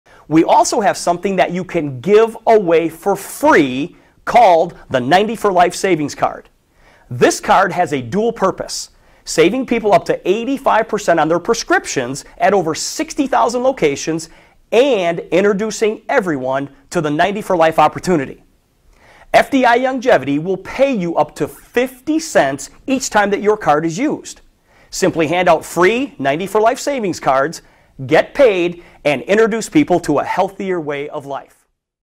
Pharmacy Discount Cards Clip
- Pharmacy Discount Card clip from the FDI-Youngevity Business Presentation.